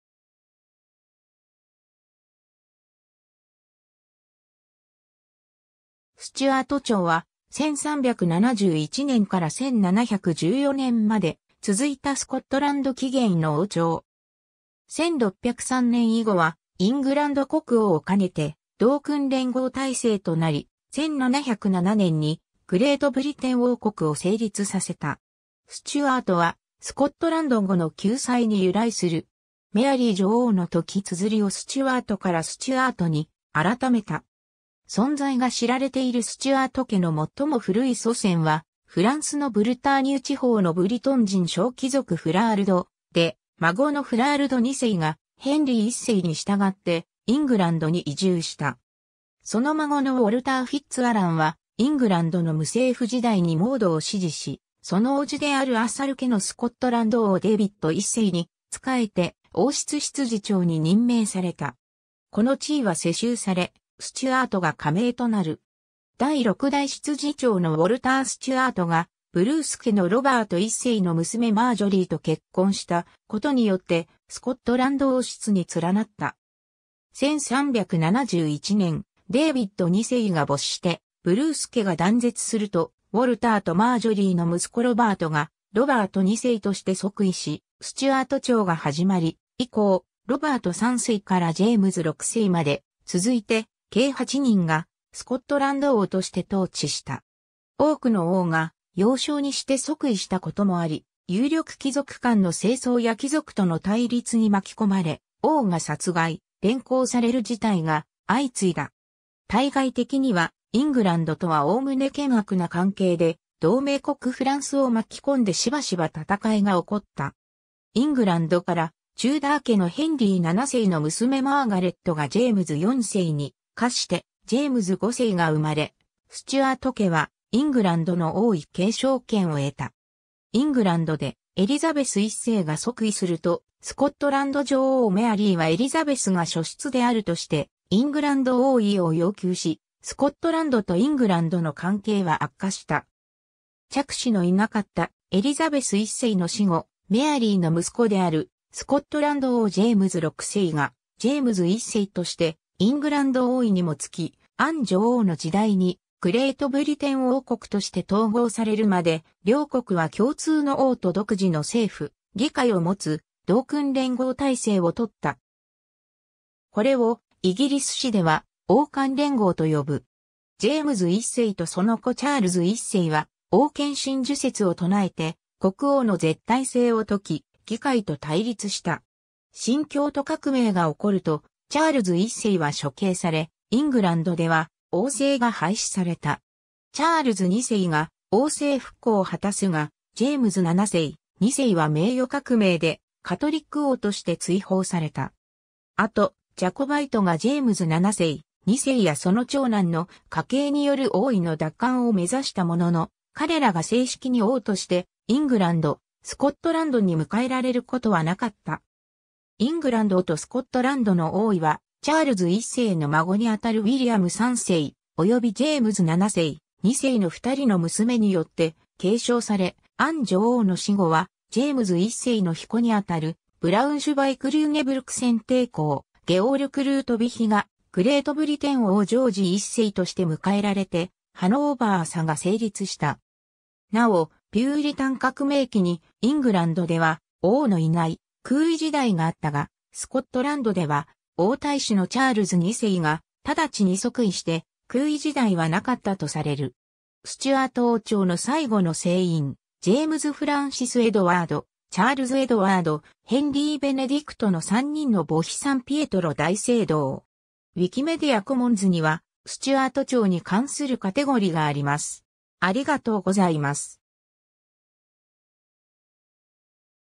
ステュアート朝
ステュアート朝（Stuart dynasty または Stewart dynasty）は、1371年から1714年まで続いたスコットランド起源の王朝。
1603年以後はイングランド国王を兼ねて同君連合体制となり、1707年にグレートブリテン王国（イギリス）を成立させた。
「ステュアート」は、スコットランド語の宮宰（Steward of Scotland）に由来する。
メアリー女王のとき綴りをStewartからStuartに改めた。
 存在が知られているステュアート家の最も古い祖先はフランスのブルターニュ地方のブリトン人小貴族フラールド（Flaald, ? - 1099年）で、孫のフラールド2世がヘンリー1世に従ってイングランドに移住した。
その孫のウォルター・フィッツアラン（? - 1177年）は、イングランドの無政府時代にモードを支持し、その叔父であるアサル家のスコットランド王デイヴィッド1世に仕えて王室執事長（Lord High Steward）に任命された。
この地位は世襲され、ステュアートが家名となる。
第6代執事長のウォルター・ステュアートがブルース家のロバート1世（母方の祖母マーガレットが第3代執事長ウォルター・ステュアートの娘である）の娘マージョリーと結婚したことによって、スコットランド王室に連なった。
 1371年、デイヴィッド2世が没してブルース家が断絶すると、ウォルターとマージョリーの息子ロバートがロバート2世として即位し、ステュアート朝が始まり、以降、ロバート3世からジェームズ6世まで、続いて計8人がスコットランド王として統治した。
多くの王が幼少にして即位したこともあり、有力貴族間の政争や貴族との対立に巻き込まれ、王が殺害・連行される事態が相次いだ。
対外的には、イングランドとはおおむね険悪な関係で、同盟国フランスを巻き込んでしばしば戦いがおこった。
 イングランドからテューダー家のヘンリー7世の娘マーガレットがジェームズ4世に嫁してジェームズ5世が生まれ、ステュアート家はイングランドの王位継承権を得た（ただし、ジェームズ4世の祖父ジェームズ2世はヘンリー7世の大おばジョーン・ボーフォートの子であり、すでにイングランド王家との血のつながりはあった）。
 イングランドでエリザベス1世が即位すると、スコットランド女王メアリー（ジェームズ5世の長女）はエリザベスが庶出であるとしてイングランド王位を要求し、スコットランドとイングランドの関係は悪化した。
嫡子のいなかったエリザベス1世の死後、メアリーの息子であるスコットランド王ジェームズ6世がジェームズ1世としてイングランド王位にも就き、アン女王の時代にグレートブリテン王国として統合されるまで、両国は共通の王と独自の政府・議会を持つ同君連合体制をとった。
これをイギリス史では王冠連合 (Union of the Crowns) と呼ぶ。
 ジェームズ1世とその子チャールズ1世は、王権神授説を唱えて国王の絶対性を説き、議会と対立した。
清教徒革命（イングランド内戦）が起こるとチャールズ1世は処刑され、イングランドでは王制が廃止された。
チャールズ2世が王政復古を果たすが、ジェームズ7世／2世は名誉革命でカトリック王として追放された。
後、ジャコバイトがジェームズ7世／2世やその長男の家系による王位の奪還を目指したものの、彼らが正式に王としてイングランド、スコットランドに迎えられることはなかった。
イングランドとスコットランドの王位はチャールズ1世の孫に当たるウィリアム3世、およびジェームズ7世／2世の2人の娘によって継承され、アン女王の死後はジェームズ1世の曾孫に当たるブラウンシュヴァイク＝リューネブルク選帝侯（ハノーファー選帝侯）ゲオルク・ルートヴィヒがグレートブリテン王ジョージ1世として迎えられてハノーヴァー朝が成立した。
 なお、ピューリタン革命期にイングランドでは王のいない空位時代があったが、スコットランドでは王太子のチャールズ2世がただちに即位して空位時代はなかったとされる。
 スチュアート王朝の最後の成員（ジャコバイトの王）ジェームズ・フランシス・エドワード（ジェームズ8世ならび3世）、チャールズ・エドワード、ヘンリー・ベネディクトの3人の墓碑サンピエトロ大聖堂（バチカン）。
   ウィキメディア・コモンズには、ステュアート朝に関するカテゴリがあります。